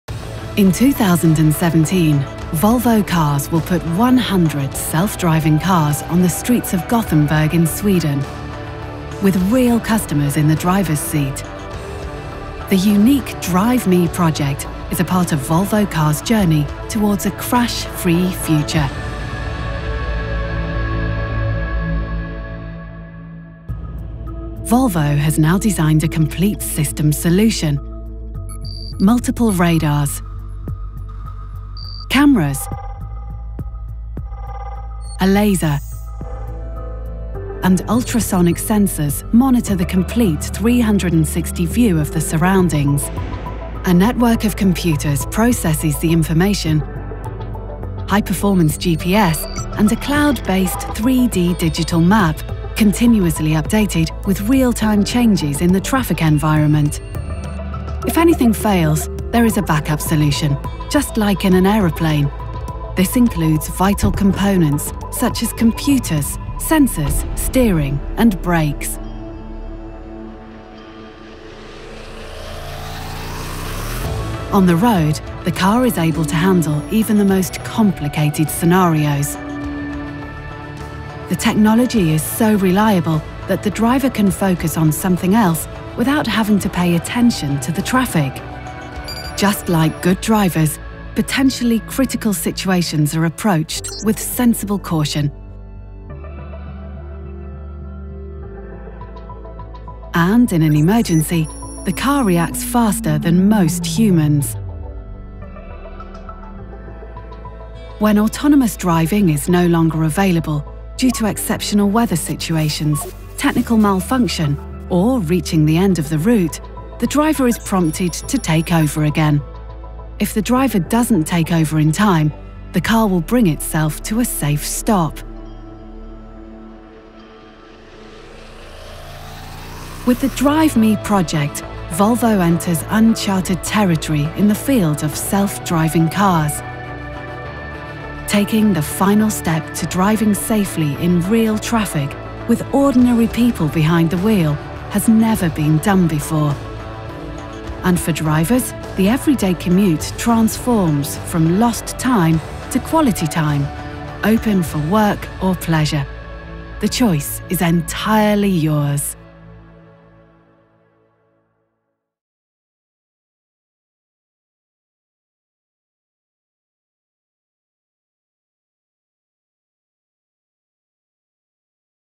Autonomous drive XC90 2015.
The video describes the technology solution Volvo Cars has choosen to their autonomous test fleet within the Drive Me project. The video consists of running footage of a Drive Me test car and animations of the different sensors and functionalities. With voiceover.